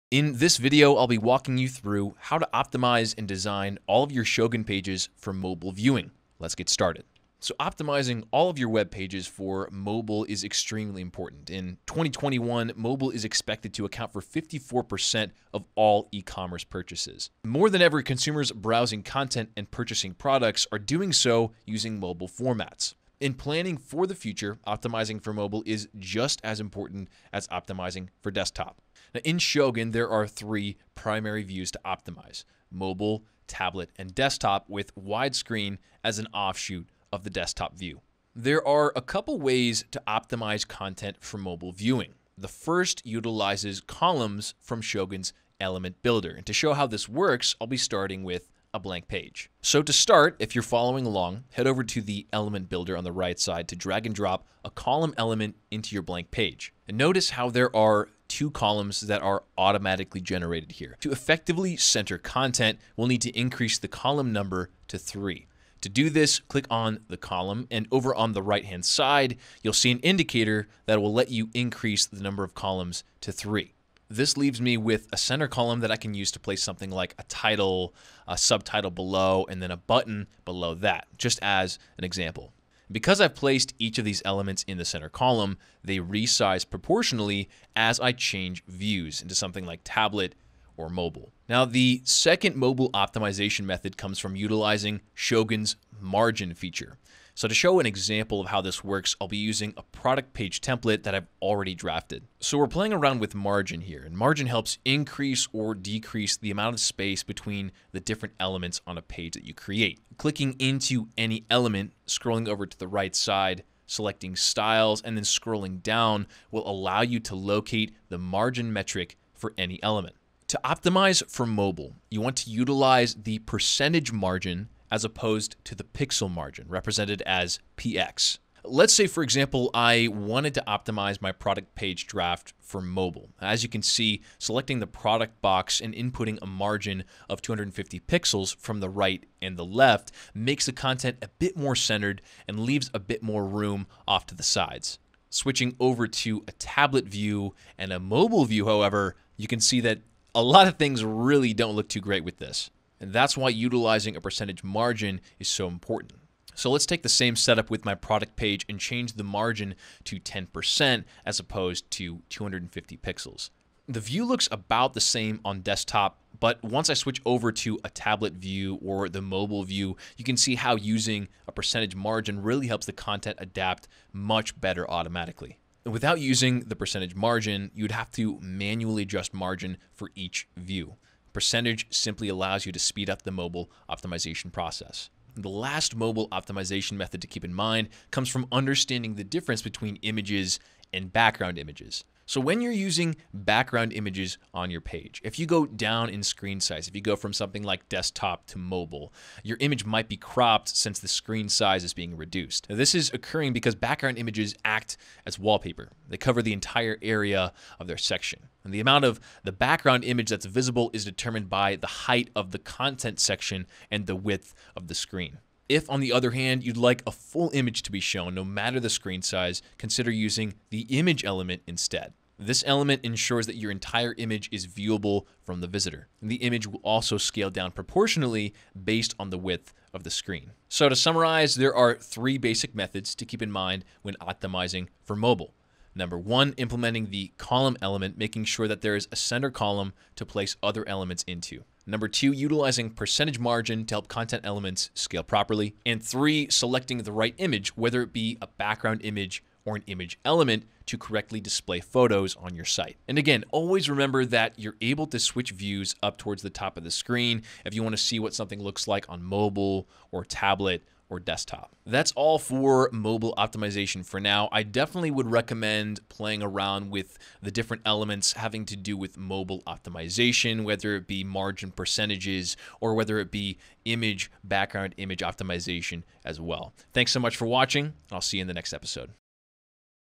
Optimizing your Ecommerce Pages for Mobile with Shogun
Optimize your ecommerce page design for mobile using Shogun's built-in mobile view functionality. Learn how to optimize your images and use universal margins to design pages that look good on all devices.